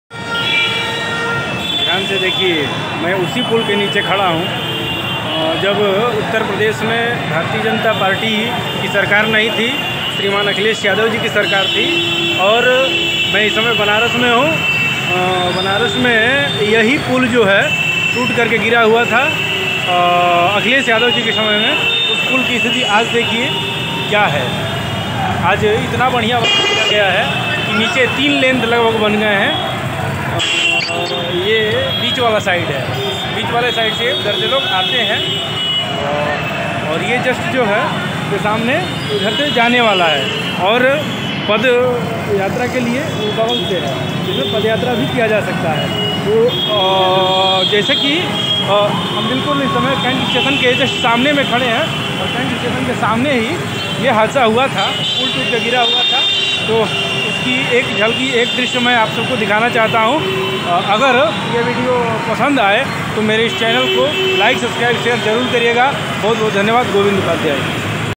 अखिलेश यादव की सरकार में जो पुल टूटा था आज की उसकी स्थिति क्या है जरूर देखें वाराणसी At Short Videos
यह वही बनारस का फ्लाई ओवर ब्रिज है जो अखिलेश यादव की सरकार में बन रहा था शिवपाल यादव जी के देख रेख में टूट गया था तब तात्कालिक उप मुख्यमंत्री श्री केशव प्रसाद मौर्य जी देख रख में बना है आज यहां का वातावरण कुछ ऐसा है कि उप्र दो लेंथ आना/जाना और नीचे से तीन लेंथ आना /जाना/ पैदल मार्ग बना हुए है पहिले एक खंभा था और आज दो खंभो से बना लगभग तीन किलमीटर से भी ज्यादा लंबा पुल बना है आप देख सकते है इसमें कोई विडम्बना नहीं है

वाराणसी से गोविन्द उपाध्याय की रिपोर्ट